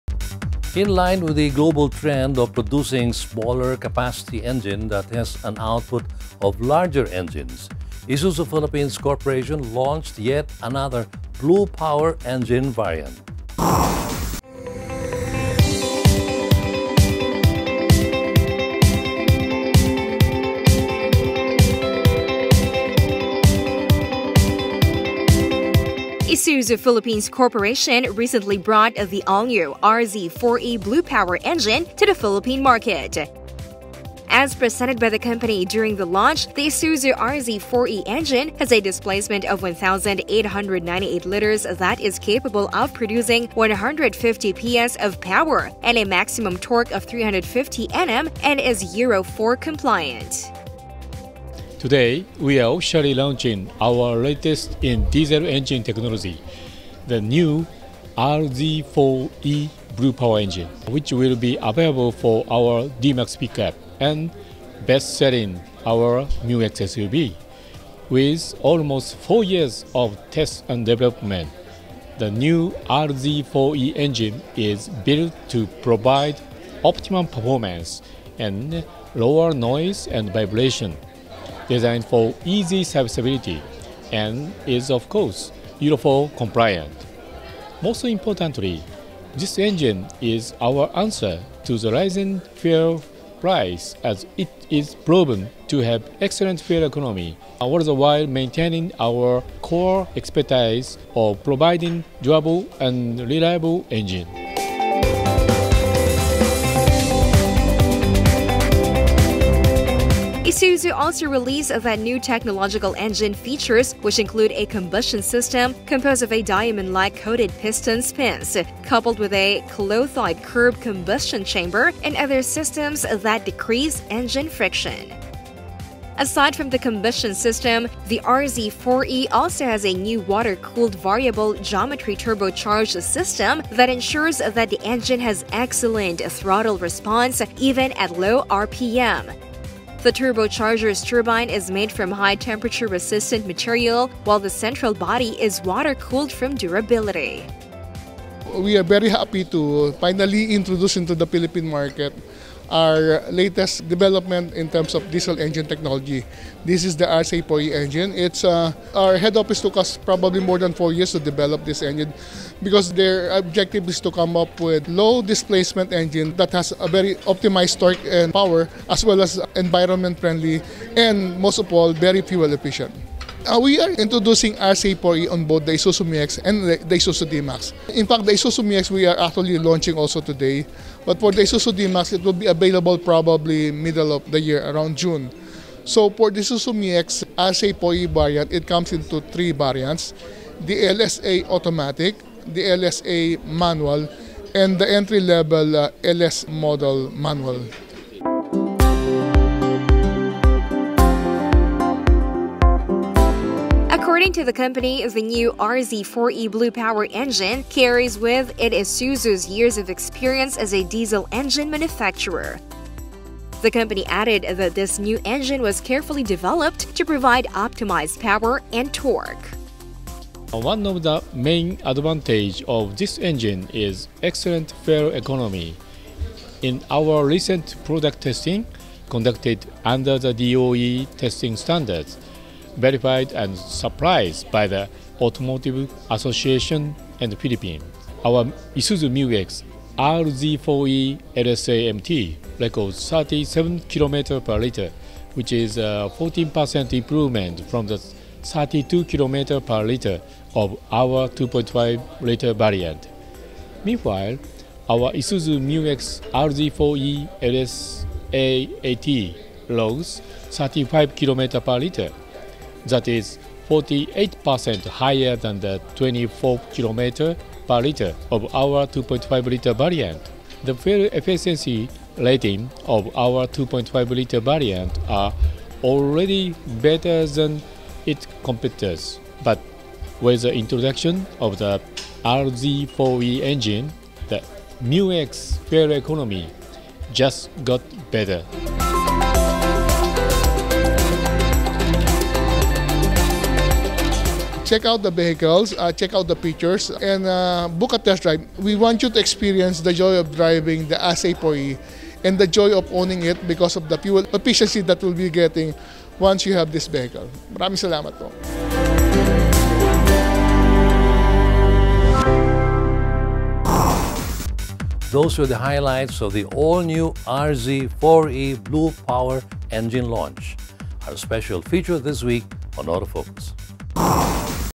Isuzu RZ4E Blue Power Engine Launch Special Feature
Isuzu Philippines Corporation recently brought the all-new RZ4E Blue Power engine to the Philippine market.

As presented by the company during the launch, the Isuzu RZ4E engine has a displacement of 1,898-liters that is capable of producing 150 PS of powerand a maximum torque of 350 Nmand is EURO 4 compliant.

Isuzu also released that new technological engine features which include a combustion system composed of a Diamond Like Coated pistons pins, coupled with a clothoid curve combustion chamber and other systems that decrease engine friction.

Aside from the combustion system, the RZ4E also has a new water-cooled Variable Geometry Turbo-charged system that ensures that the engine has excellent throttle response even at low rpm. The turbo-charger’s turbine is made from high temperature resistant material while the central body is water cooled for durability.

According to the company, the new RZ4E Blue Power engine carries with it Isuzu’s years of experience as a diesel engine manufacturer. The company added that this new engine was carefully developed to provide optimized power and torque.